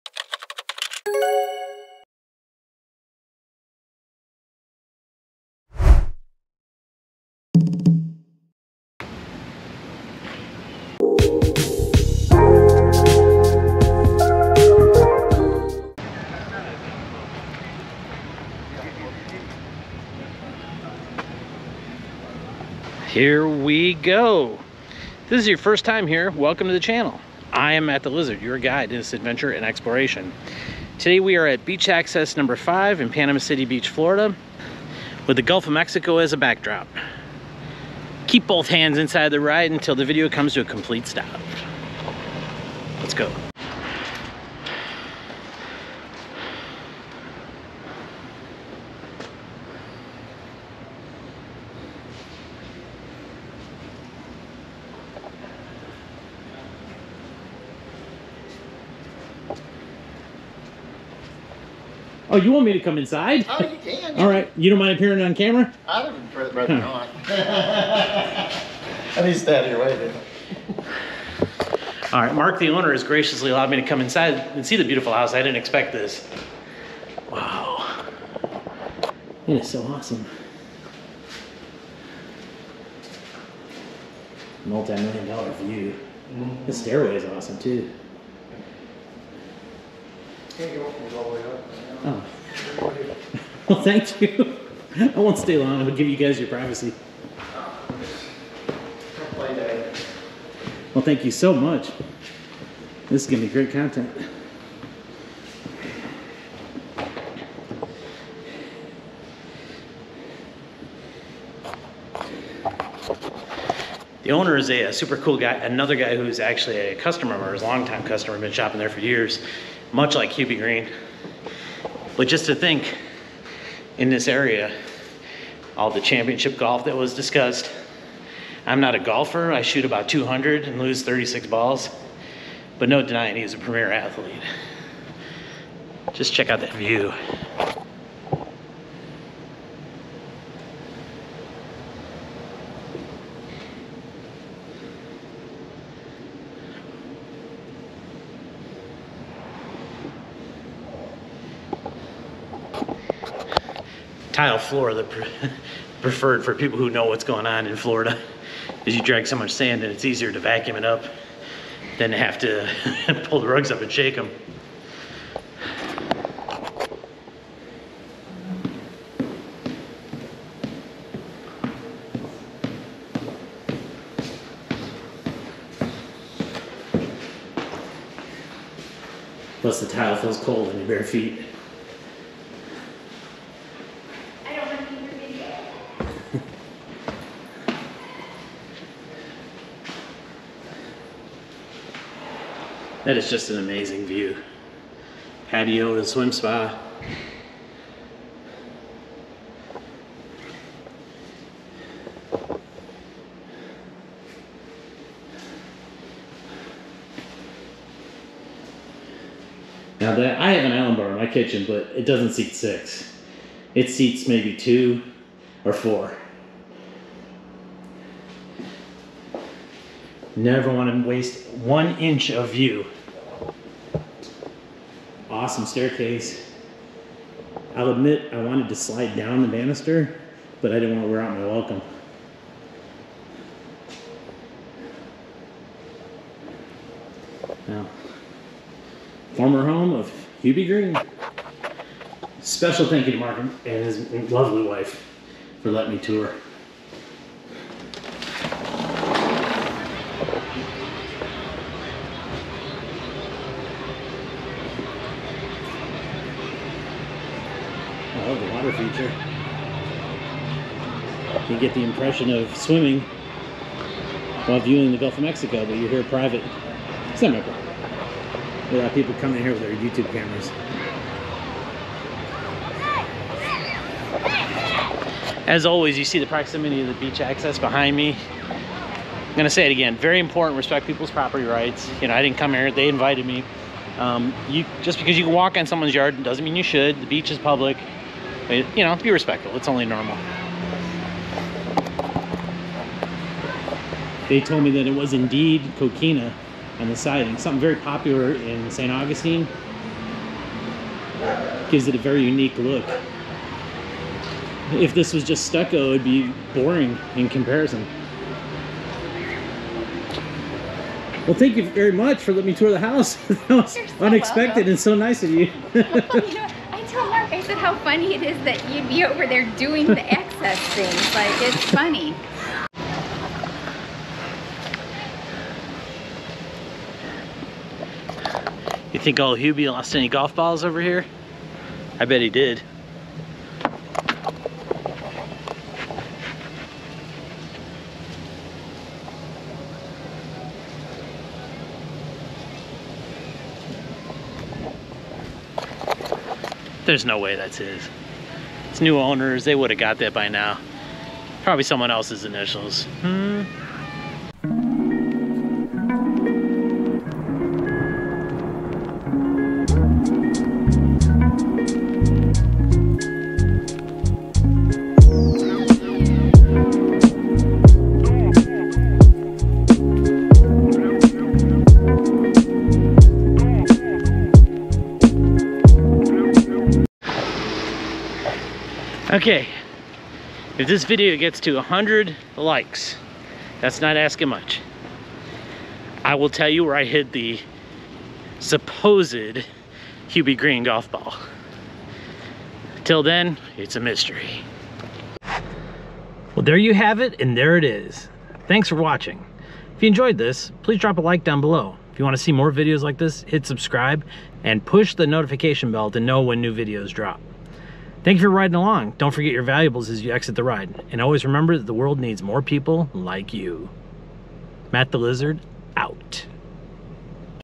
BEACH ACCESS 5 | PANAMA CITY BEACH | FLORIDA
A continuation of the Beach Access series, this time we get to tour the former home of Pro Golfer Hubie Green.